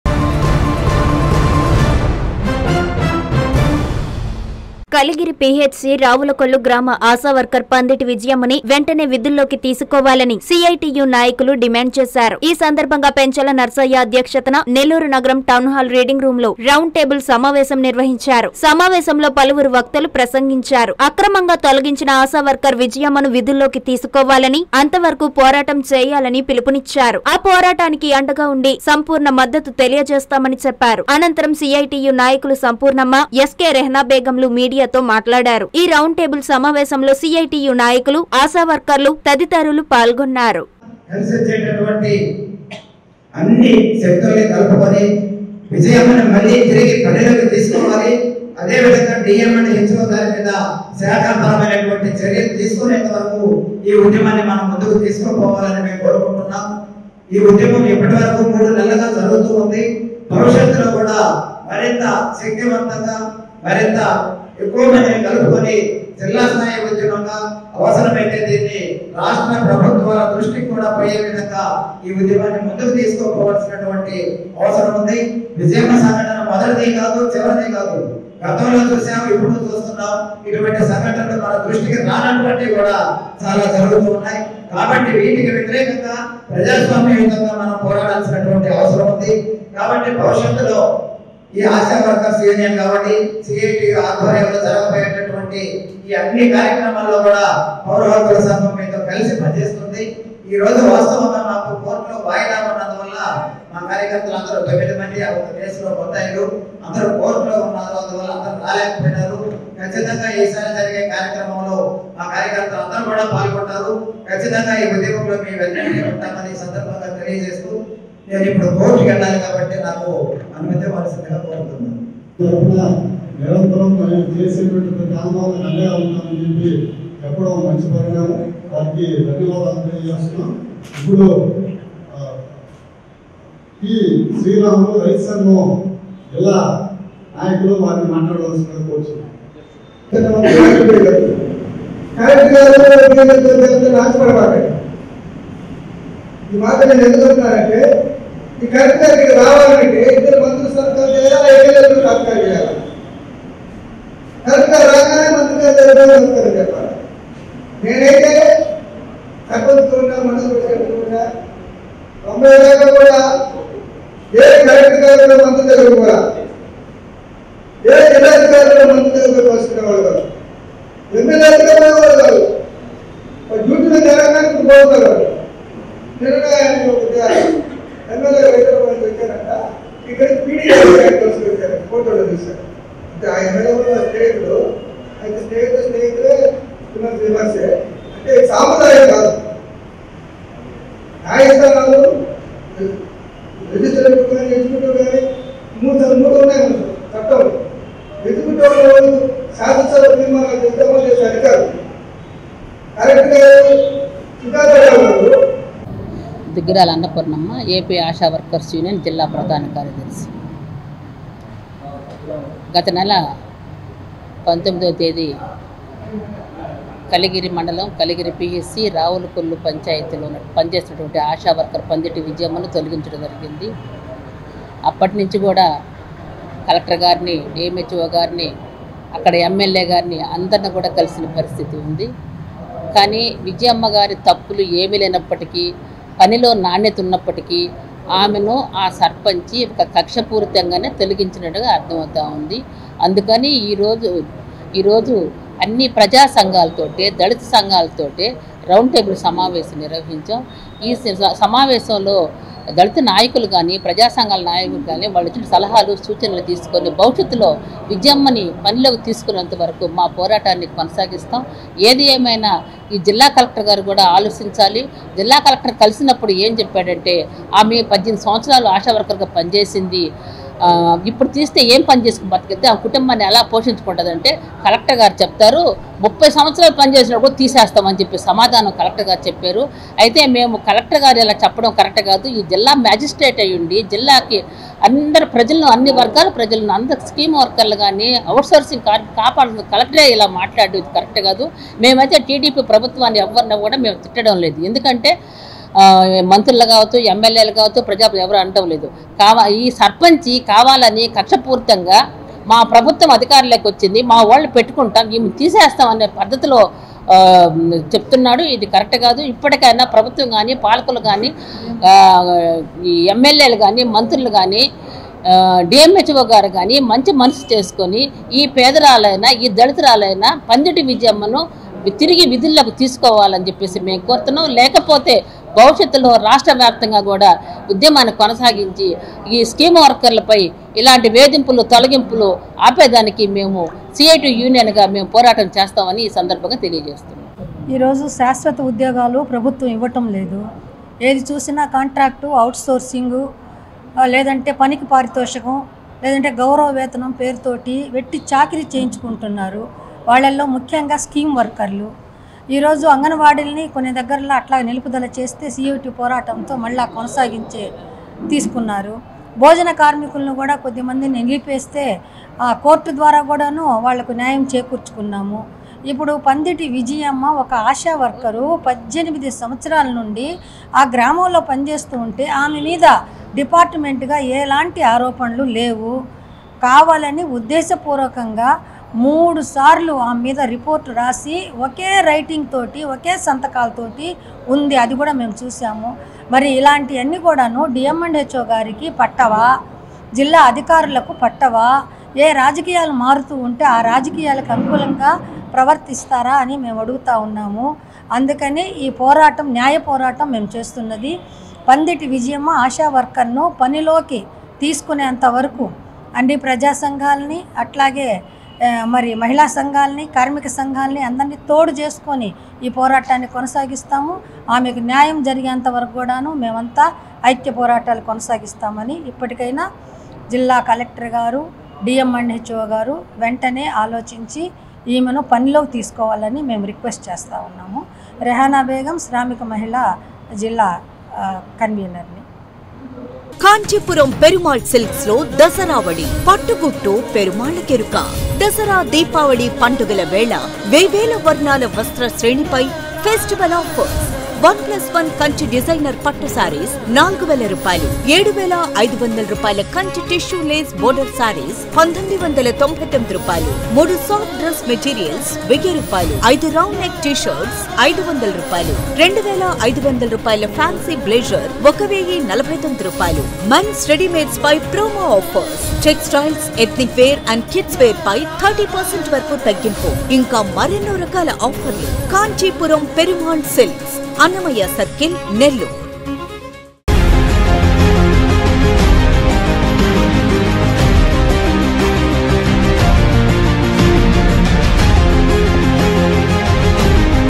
విజ‌య‌మ్మ‌ని వెంట‌నే విధుల్లోకి తీసుకోవాలి
- సీఐటీయూ డిమాండ్‌
- టౌన్ హాల్ రీడింగ్ రూమ్‌లో రౌండ్ టేబుల్ స‌మావేశం